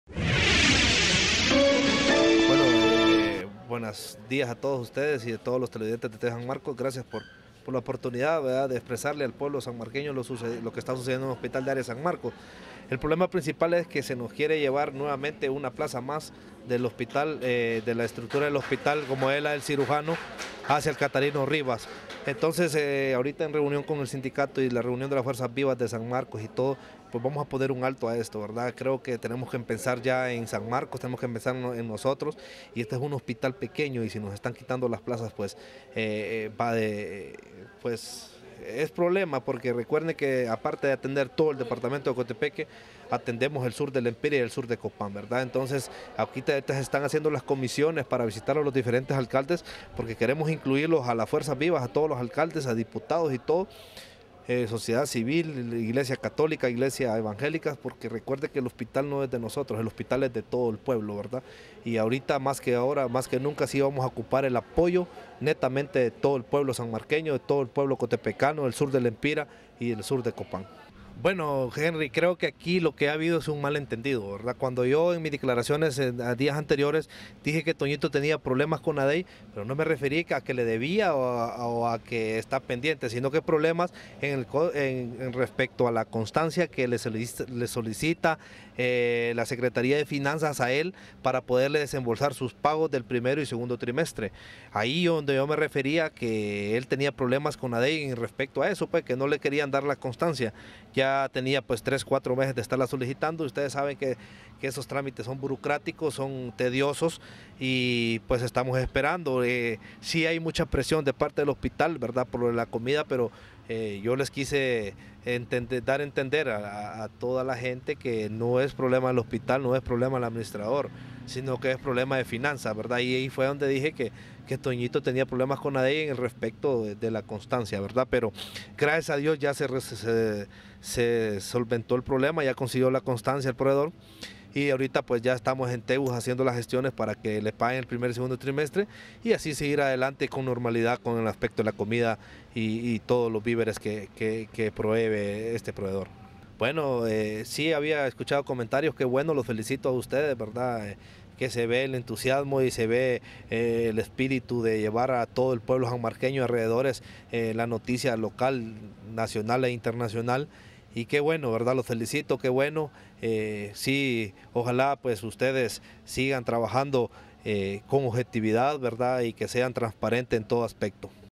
Avance noticioso sanmarcostv_sept 09_edición 3
El Licdo. Yariel López administrador del hospital de San Marcos nos habla de algunas dificultades que se suscitan en dicha institución...Síguenos